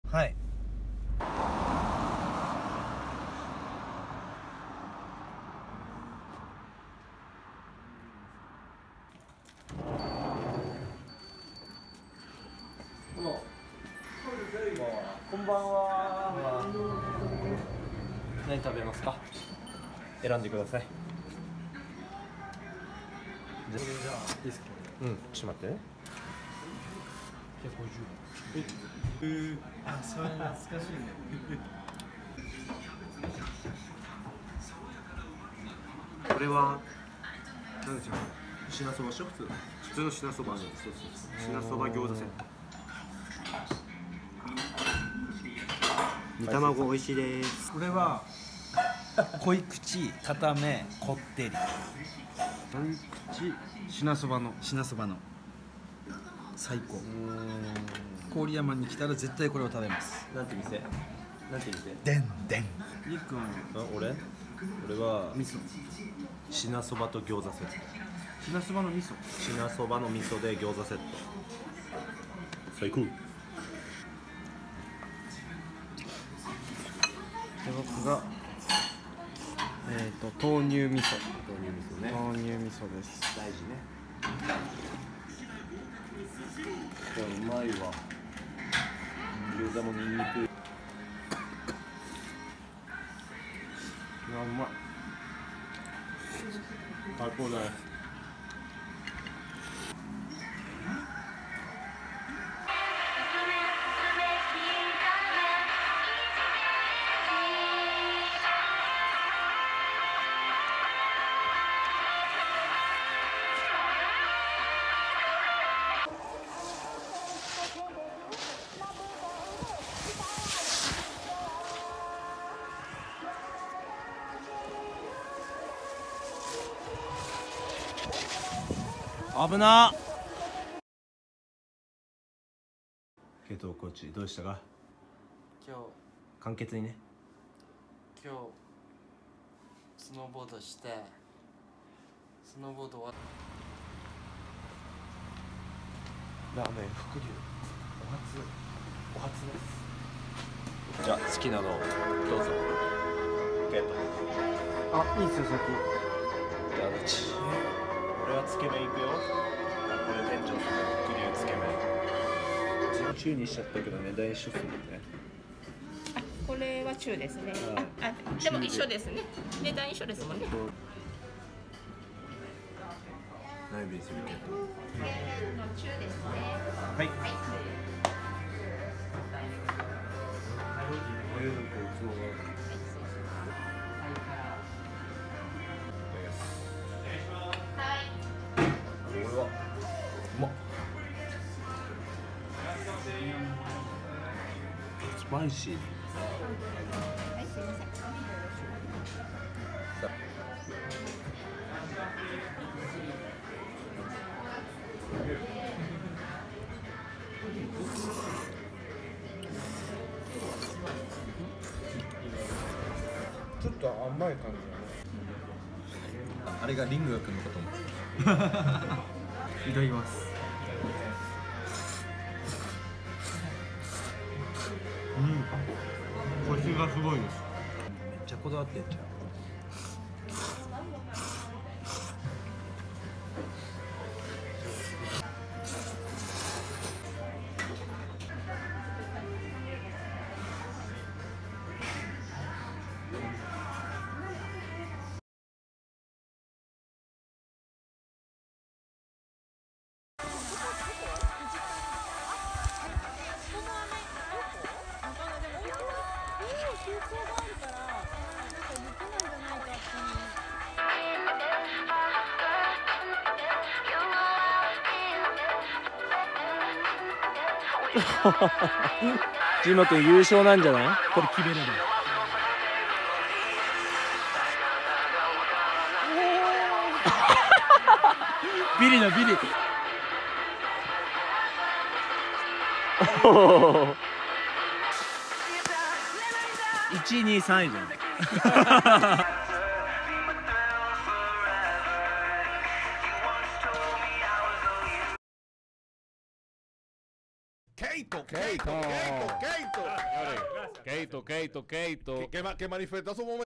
らーめん スノーボーダーいきつけ 福島県の おすすめらーめん（でんでん）（伏竜） 塩山陸 ジーマくん
スノーボード映像場所　ハーフパイプ（ハンターマウンテン）
ラーメン映像場所　１.でんでん　２.伏竜
塩谷　陸　しおやりく　スノーボーダー
中島　のりやす　ジーマ　スノーボーダー
熊崎　圭人　くまざきけいと　スノーボーダー
あだち　だいき　トレーナー　郡山市とうばい接骨院